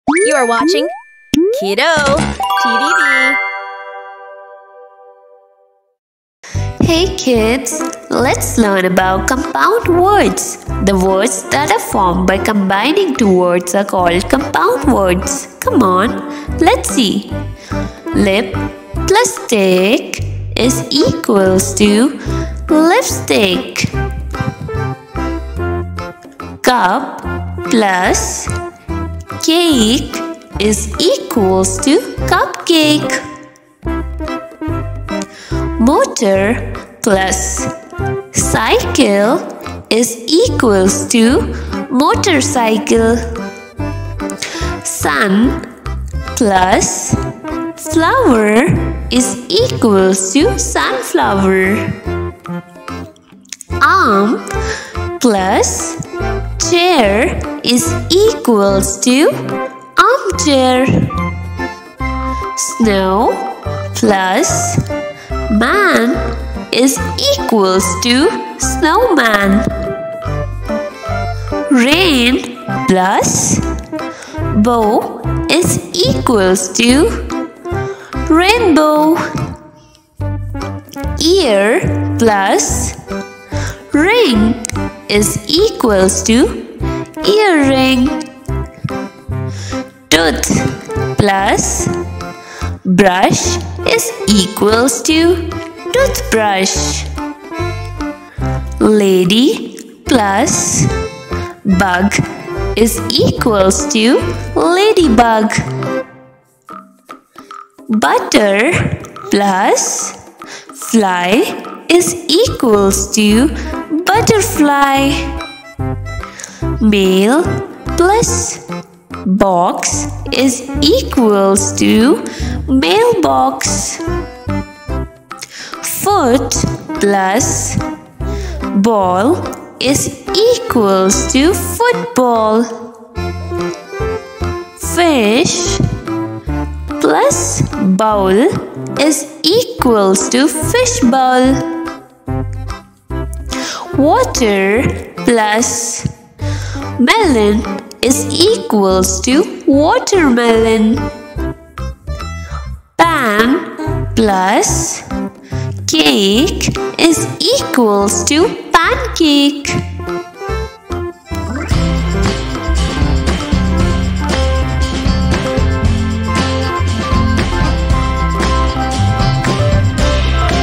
Let's Play Word Detective! Fun with Compound Words for Kids!
Hey KiddoTVvv friends! Get ready to unleash your inner word detective with this exciting adventure into the world of compound words!

In this playful video, we'll discover how two separate words can magically transform into one totally new and awesome word!

Remember, learning is way more fun when it's filled with laughter and surprises!  So, grab your thinking caps and let's dive into the amazing world of compound words together!